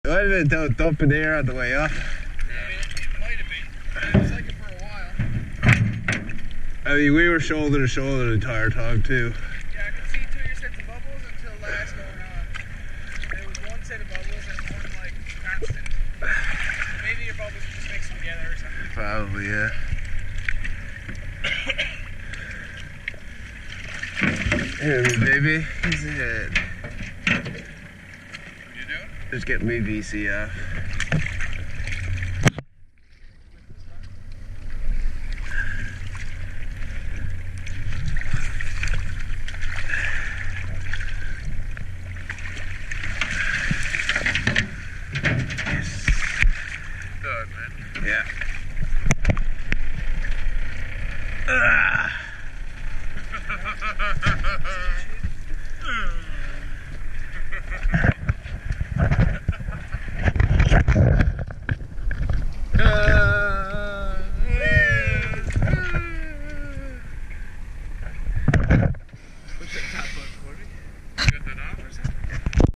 On the hunt for Lamp Shell Brachiopods (4 of 4)
Heading down the steep slope of Gadds Harbour to 100ft looking for some Brachiopods